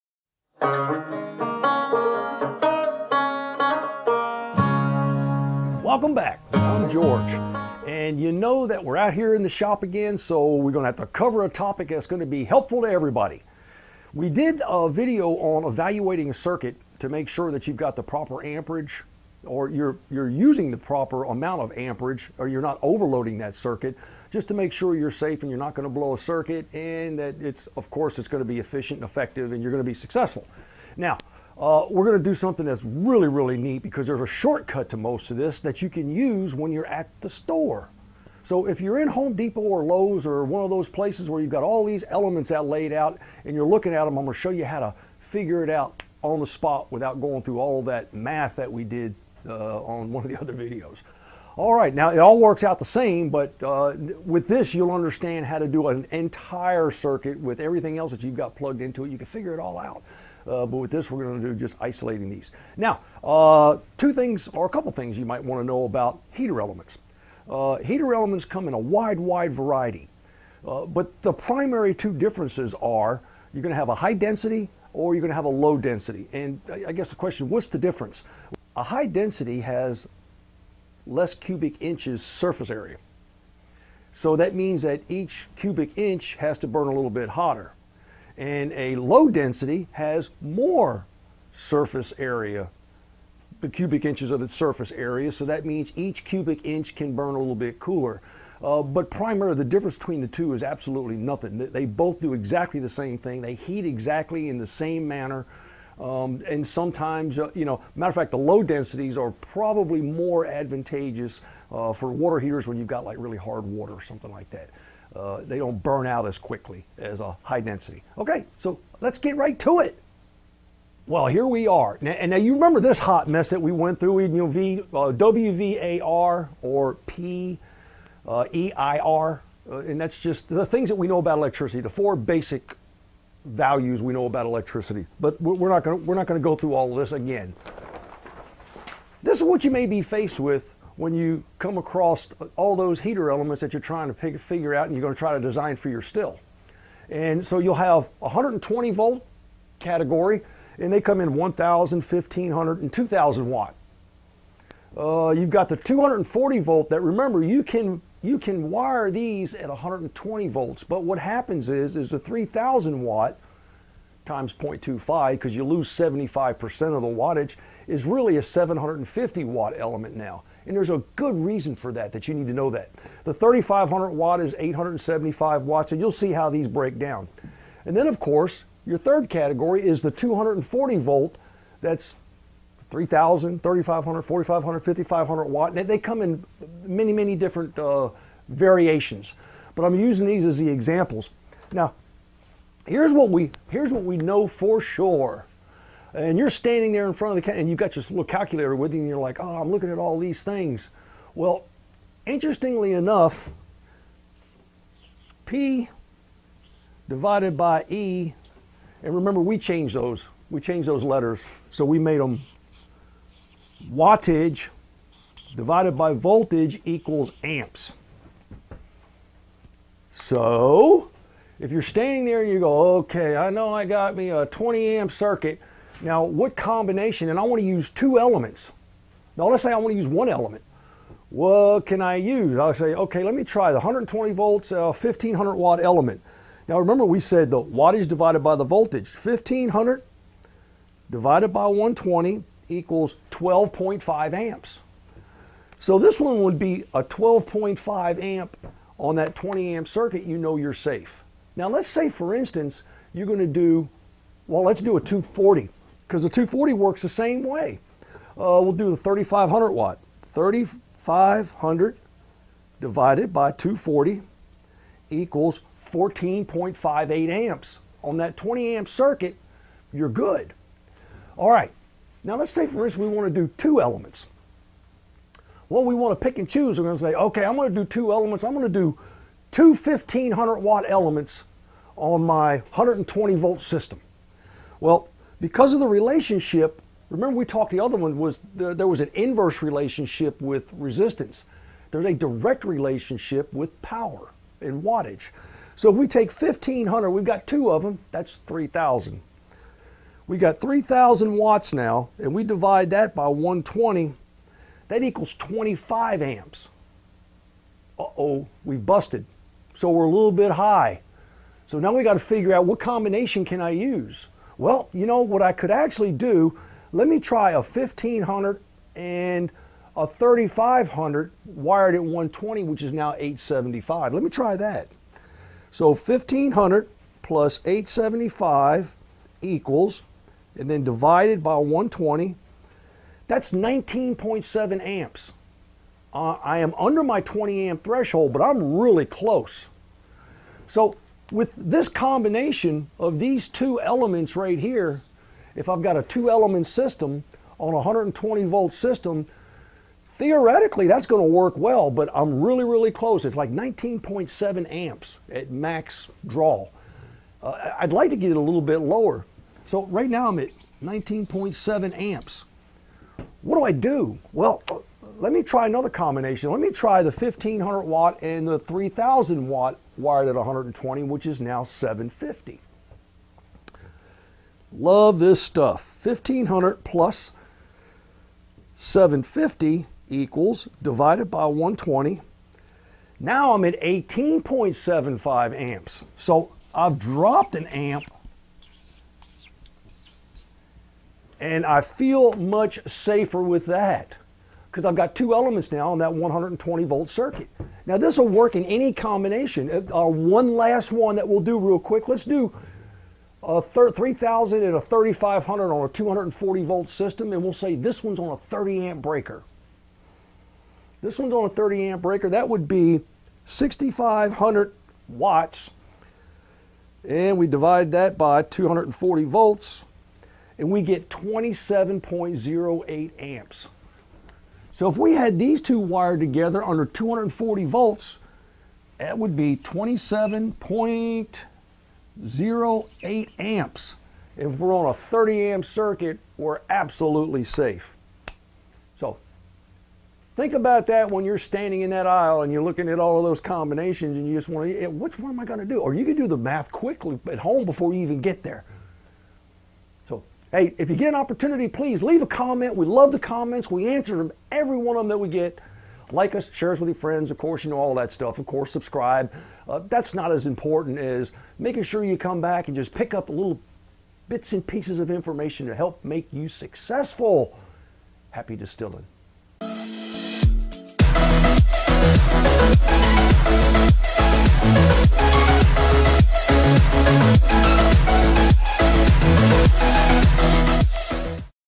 SELECTING THE PROPER HEATING ELEMENT SIMPLIFIED
When faced with the selection of elements this will help you decide which one or combination will be safe and you will be successful.